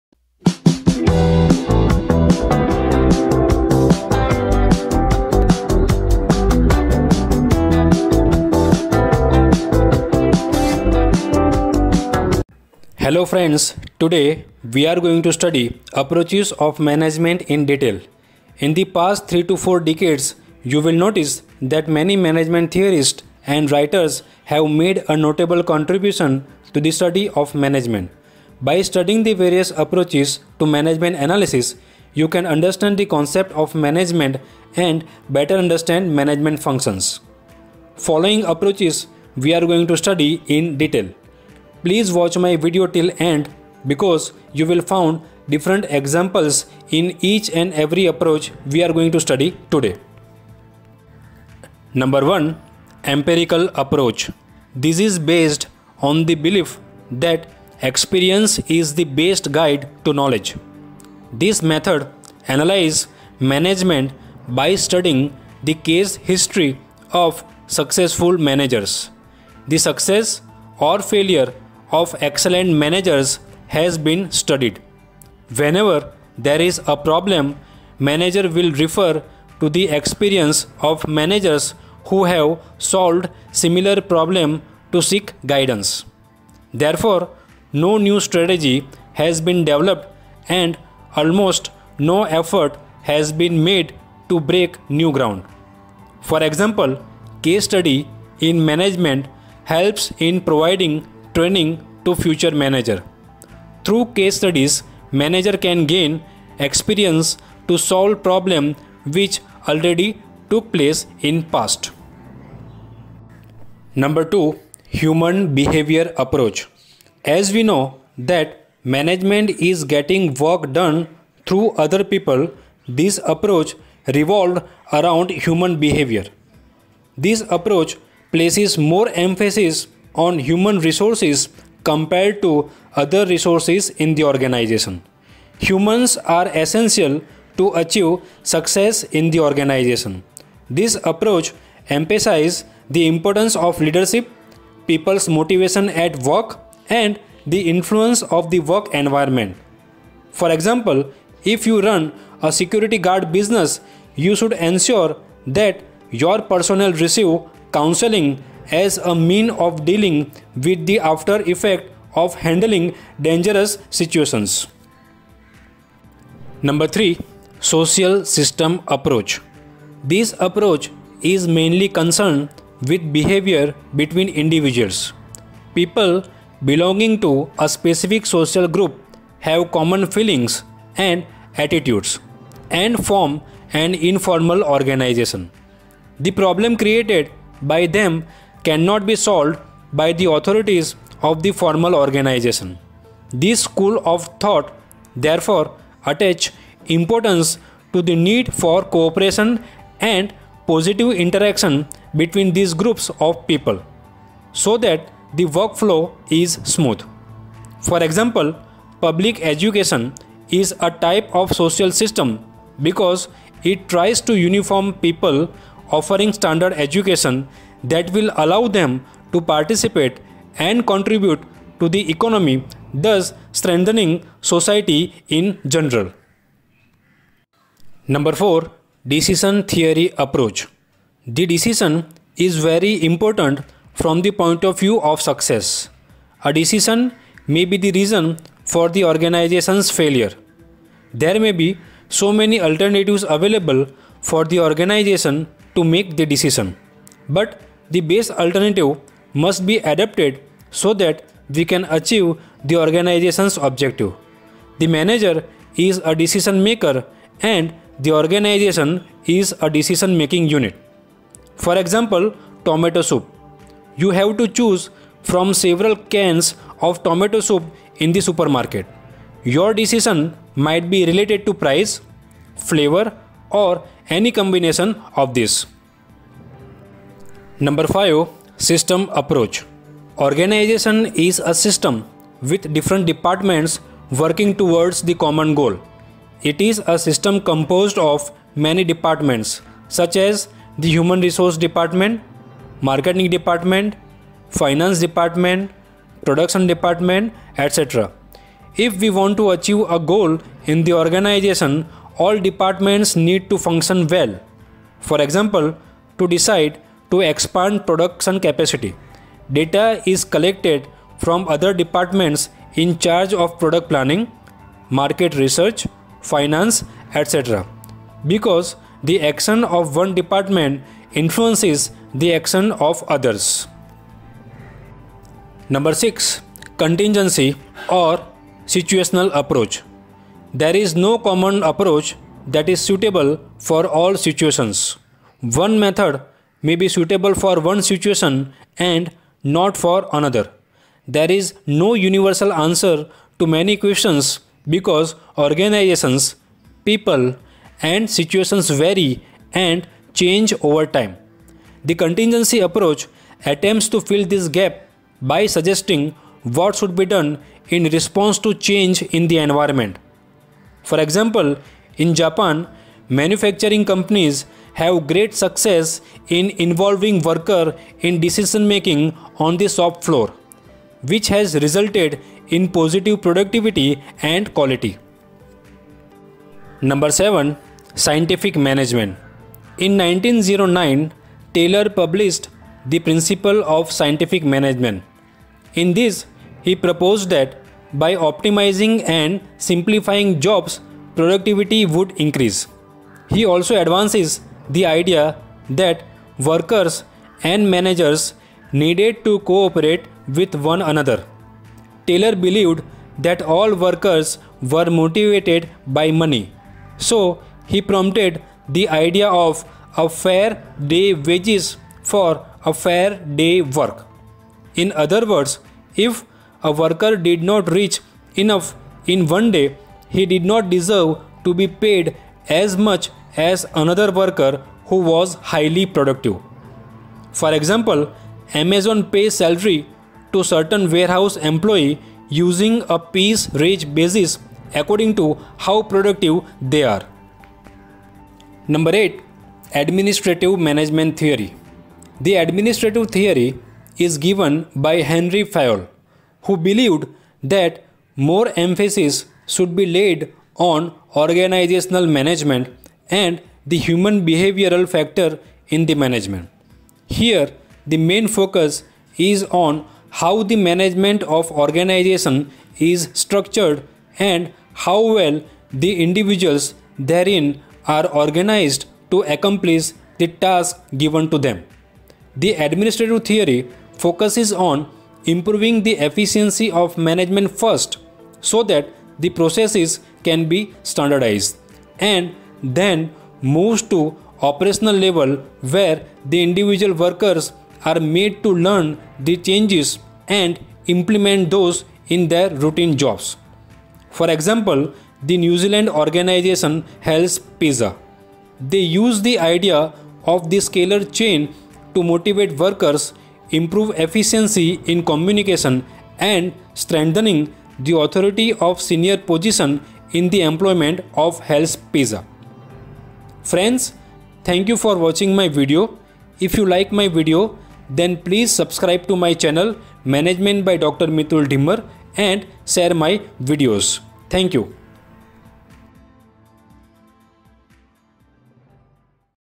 Approaches of management with examples / Different schools of management thought with examples
Hello friends.
In this video I had explained different approaches of management with different examples in detail.

This video is helpful for CBSE business students class 12, BBA and MBA students and all other management students.

I had  covered following approaches with different examples in each and every approach.
TIMESTAMPS
0:00 Welcome to my channel Management By Dr. Mitul Dhimar
0:50 Empirical approach
1:53 Human behaviour approach
2:45 Social systems approach
3:47 Decision theory approach
4:36 System approach
5:25 Contingency or situational approach
6:16 Fredrick Taylor’s Scientific Management
7:18 The administrative management theory

I hope you enjoyed and able to clarify topic with my video of approaches of management.

Moreover I had prepared following videos on management

Majority of photographs are downloaded from freepik.com
Videvo website use for free video download